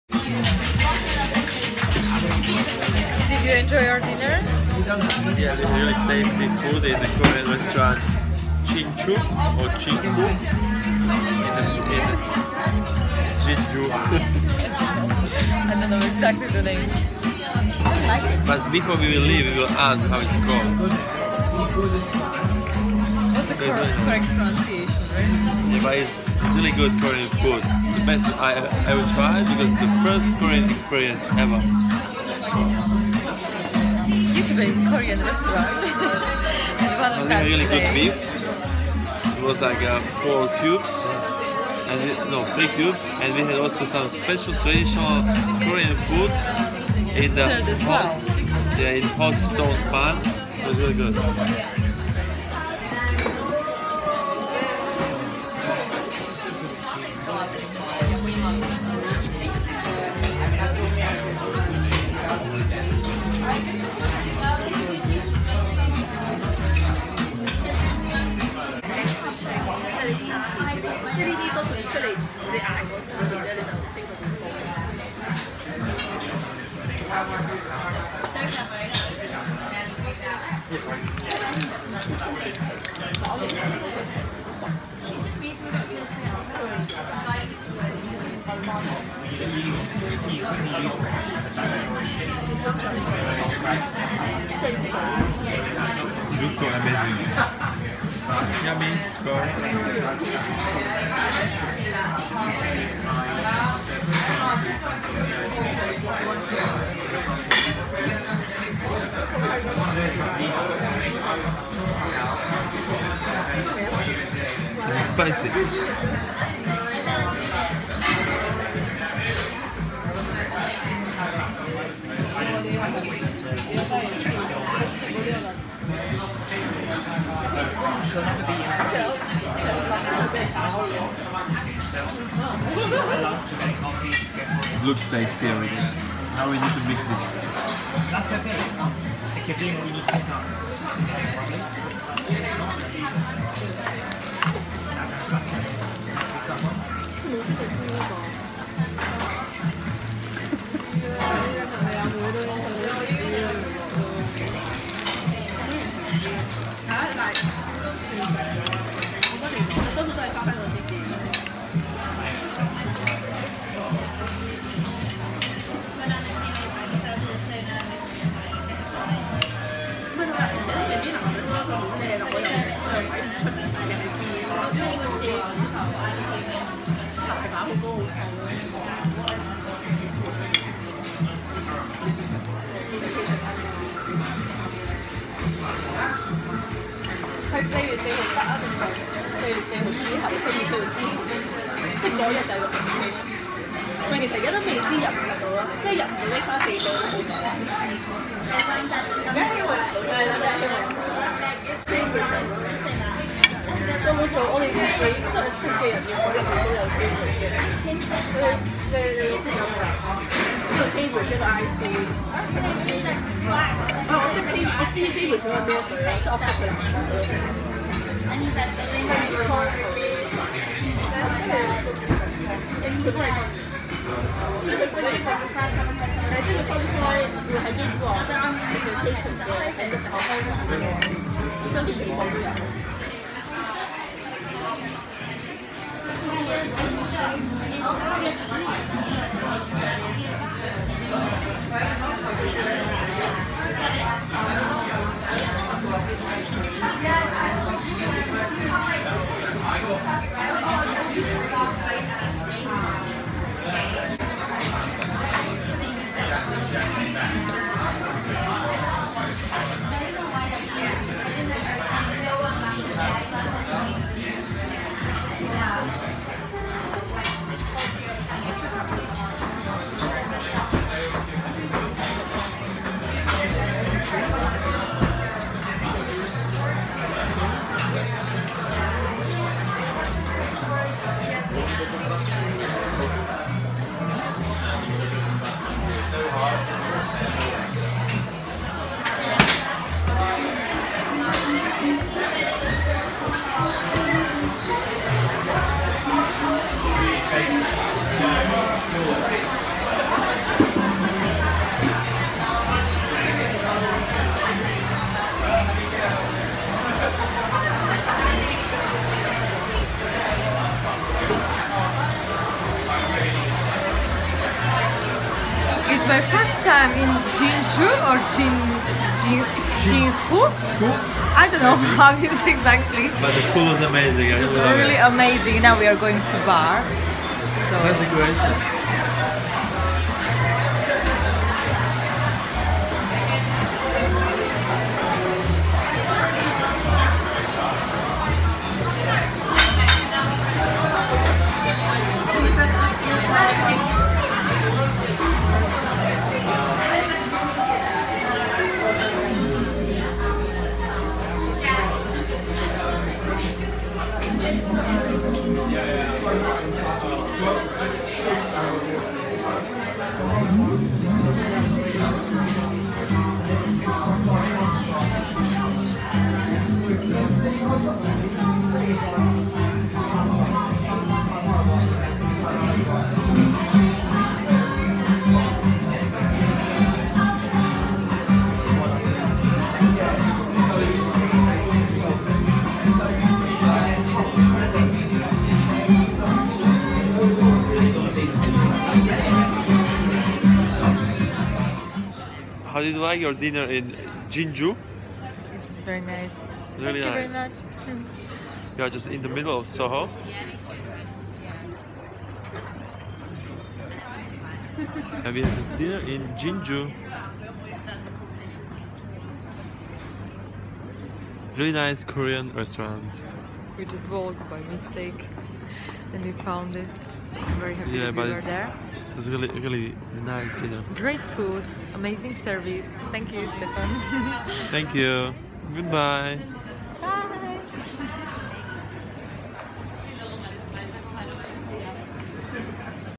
JinJuu Soho London - The Best Korean Restaurant in London
We had a great dinner in the Korean restaurant JinJuu, Kingly Court, 15 Kingly Street, Carnaby, London. JinJuu is in heart of Soho near the famous Liberty.

This was our first Korean restaurant experience. Thank Stephan for great service and recommending the tastiest dishes 😀

As a starter we ordered:

SEA-WOO POPS         £8.50
- 5 crispy fried round prawn cakes served on sticks, addictive creamy gochujang mayo on the side

KOREAN FRIED CHICKEN SLIDERS     £7.50
- 2 x golden fried chicken thighs, our signature sauces, mayo, crispy iceberg, tossed in a brioche bun

Our waiter advised as great mains:
GLAZED USDA SHORT RIBS       £26
- slow-braised, tender cubed of short rib. Served with crispy straw potato, garlic chips, grilled vegetables & blasted with our JinJuu soya glaze.

BIBIMBAP     £9.50
- served in a traditional stone bowl, filled with white rice & seasonal vegetables. Gochujang sauce on the side. Topped with a fried egg.

At the end we had a delicious dessert:
SNICKERS HOTTEOK     £7
- flat Korean doughnut stuffed with salted caramel & roasted peanuts, peanut parfait, chocolate ganache, praline & Nutella powder

I am forever grateful to each and every one of you for watching, commenting, and being a part of my channel. xxx